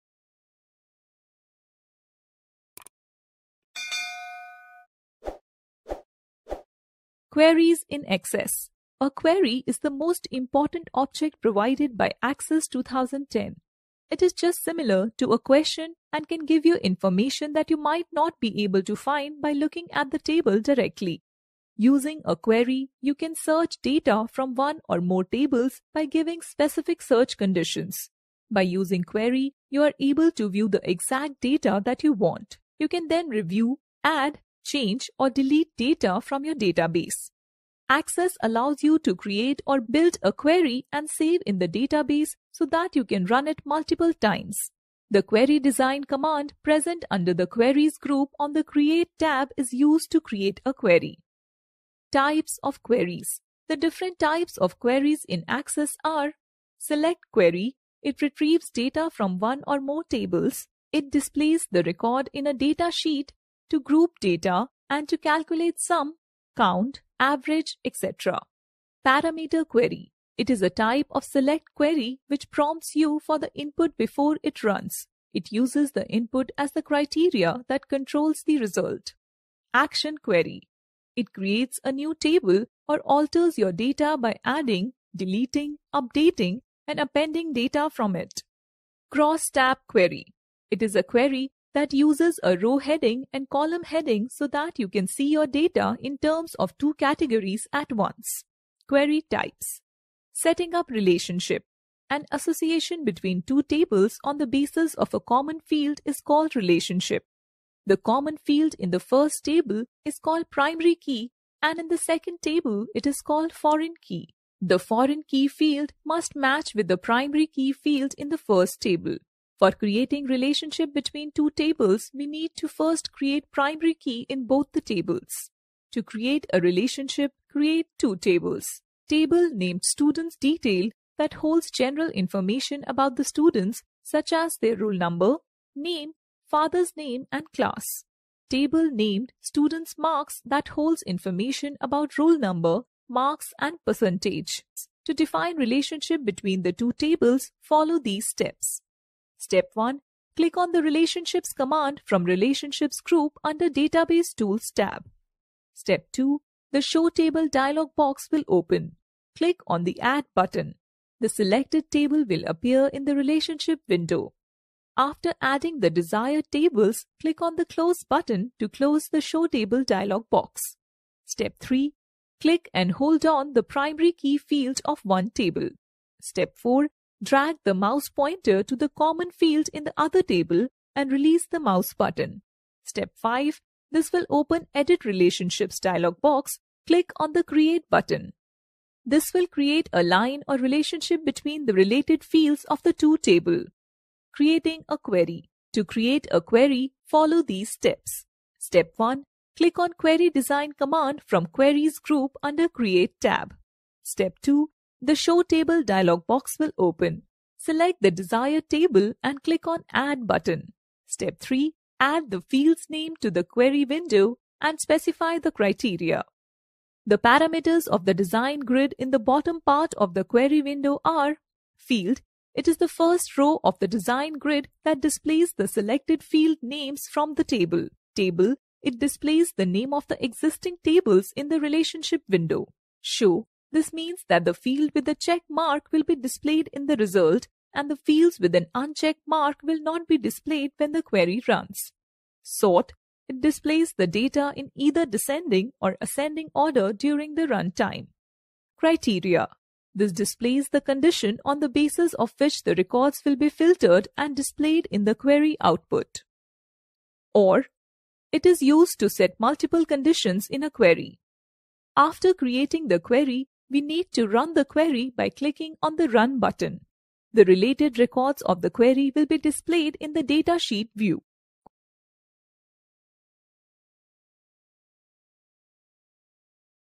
Querries in access
In this video, the basics of Querries in access will be introduced to the students.